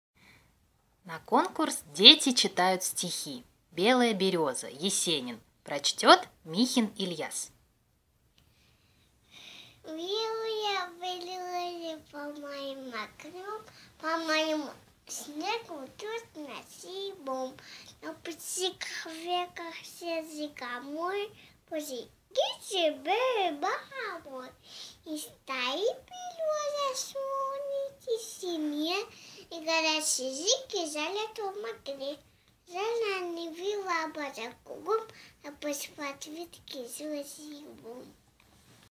Сергей Есенин «Берёза»
Ильяс  2,5 года  г.Саратов